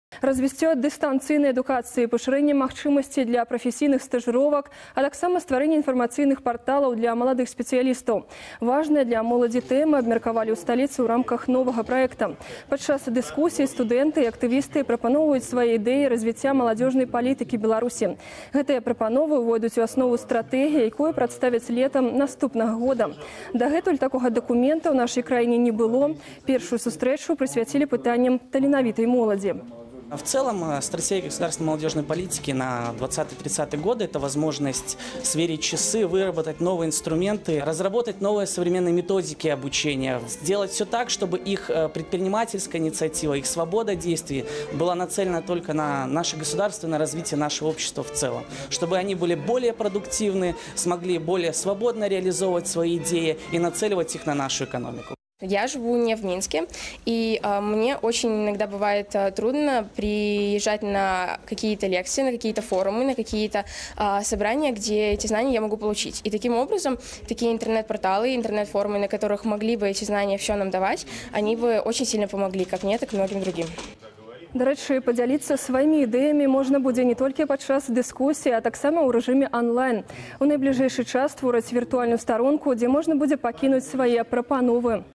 Важныя для моладзі тэмы абмеркавалі ў сталіцы ў рамках новага праекта
Развіццё дыстанцыйнай адукацыі, пашырэнне магчымасцей для прафесiйных стажыровак, а таксама стварэнне інфармацыйных парталаў для маладых спецыялістаў. Важныя для моладзі тэмы абмеркавалі ў сталіцы ў рамках новага праекта. Падчас дыскусій студэнты і актывісты прапаноўваюць свае ідэі развіцця маладзёжнай палітыкі Беларусі. Гэтыя прапановы увойдуць ў аснову стратэгіі, якую прадставяць летам наступнага года. Дагэтуль такога дакумента ў нашай краіне не было. Першую сустрэчу прысвяцілі пытанням таленавітай моладзі. Дарэчы, падзяліцца сваімі ідэямі можна будзе не толькі падчас дыскусій. Таксама у рэжыме online. У найбліжэйшы час створаць віртуальную старонку, дзе можна будзе пакінуць свае прапановы.